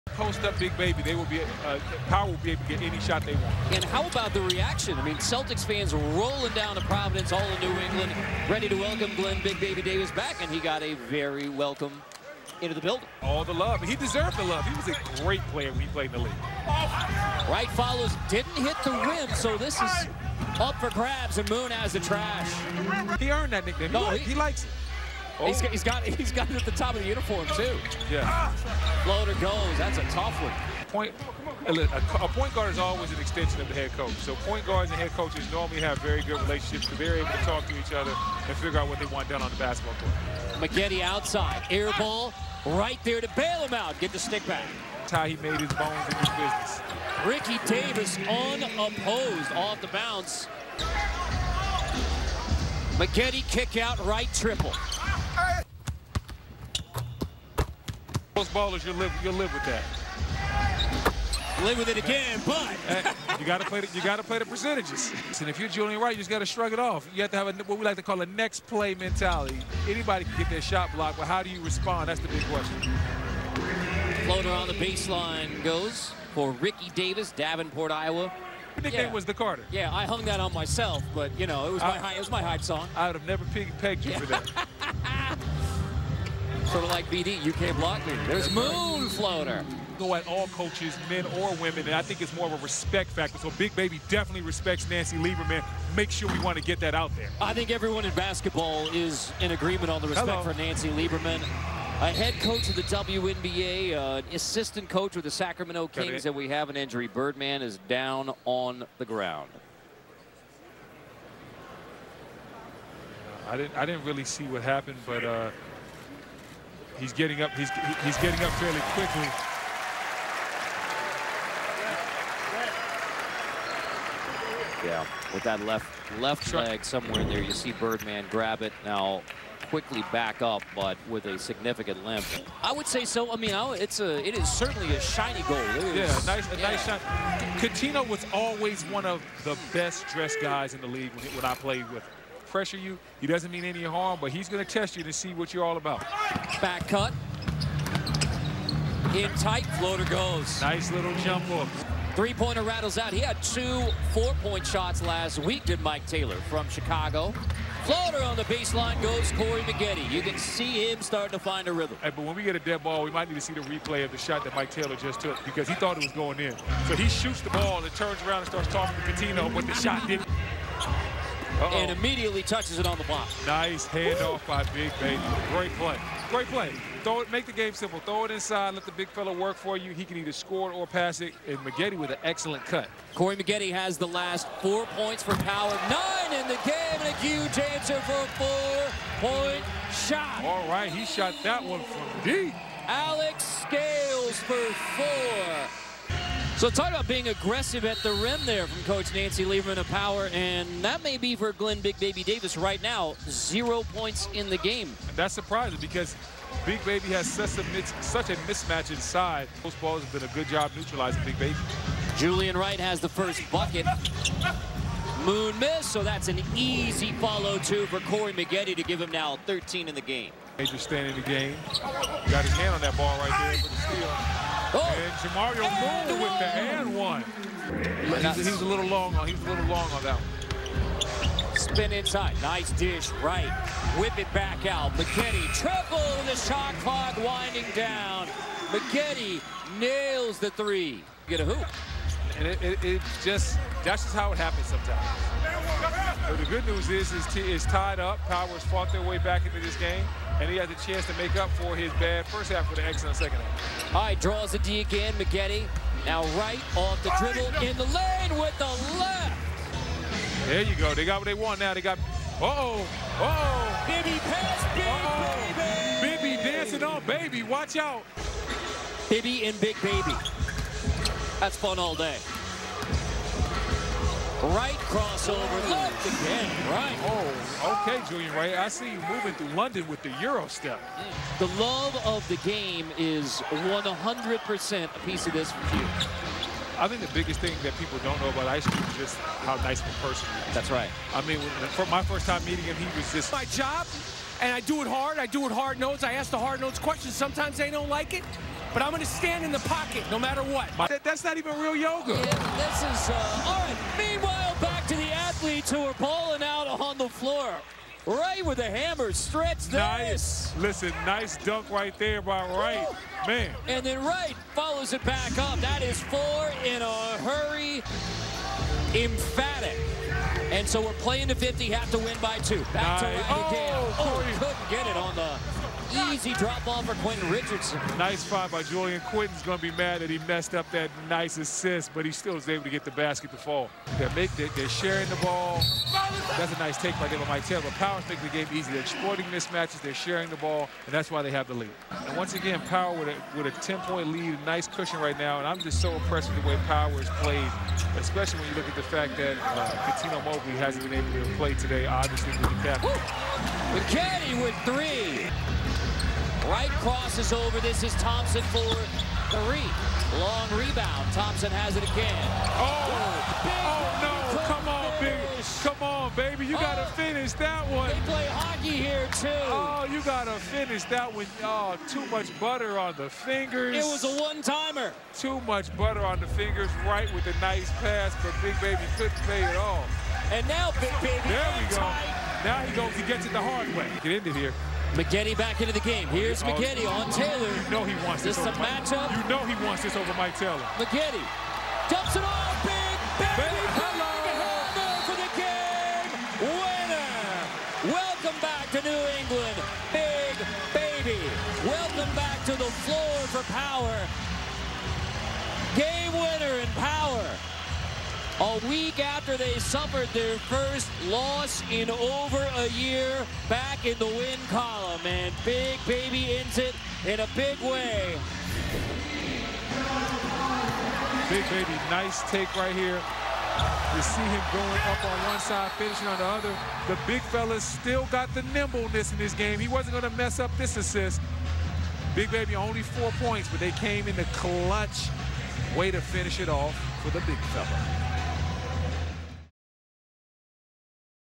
Big Baby Davis hits ANOTHER game-winner for Power | CBS Sports
Power pulls away in the second half to improve to 3-1 with a victory over Ghost Ballers.